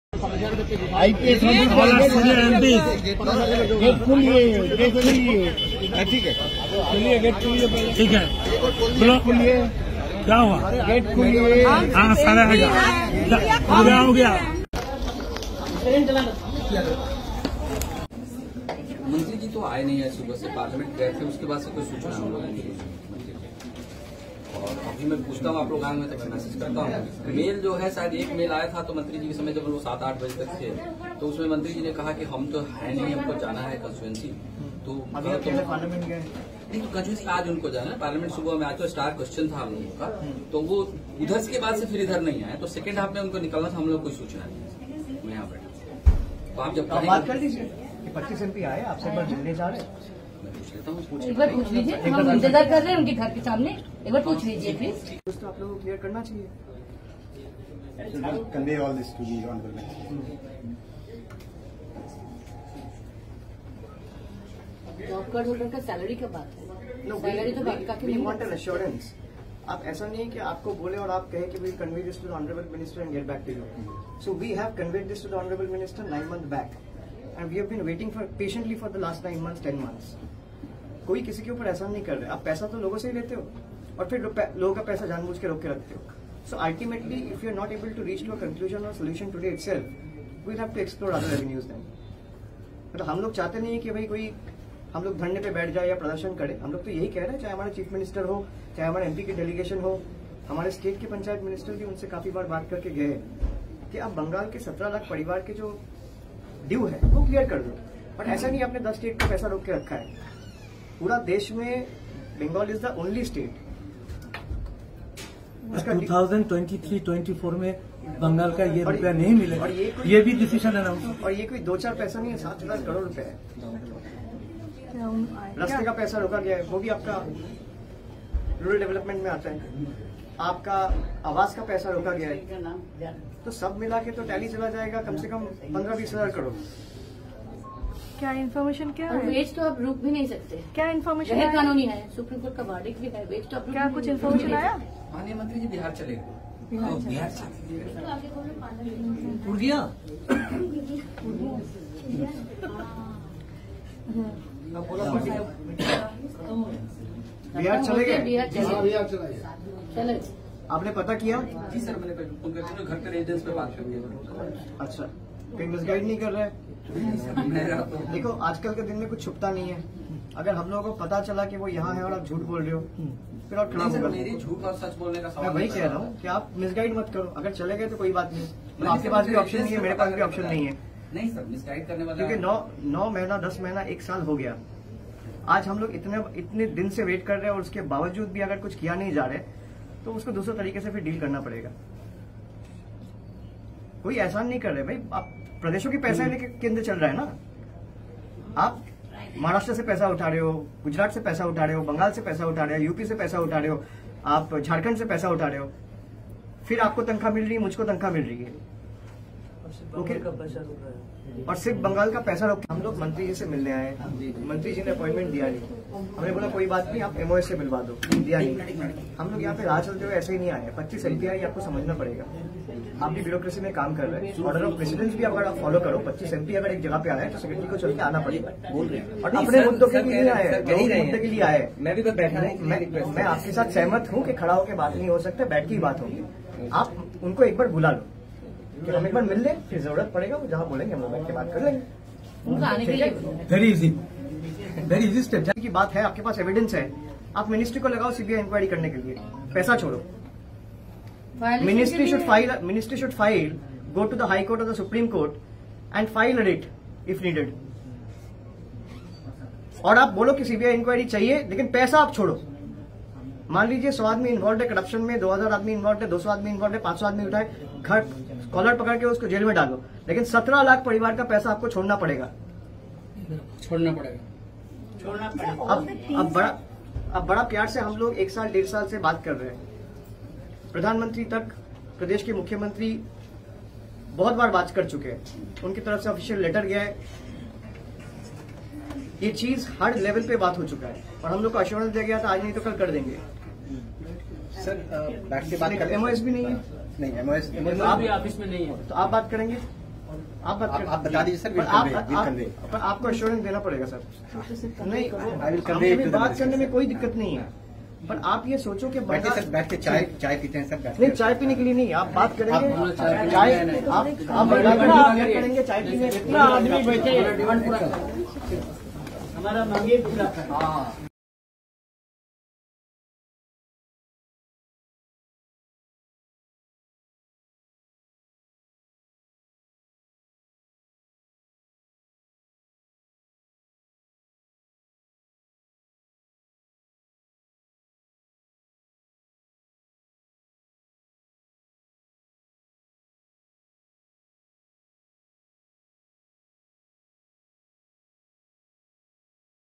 WB MPs wanted to discuss about MGNREGA due funds for Bengal with Giriraj Singh
@StateReporterINDIA : A report from Delhi on 05-04-23.
Delhi : West Bengal MPs sought an appointment with Minister of Rural Development Sri Giriraj Singh to discuss & prioritise the release of MGNREGA funds for Bengal.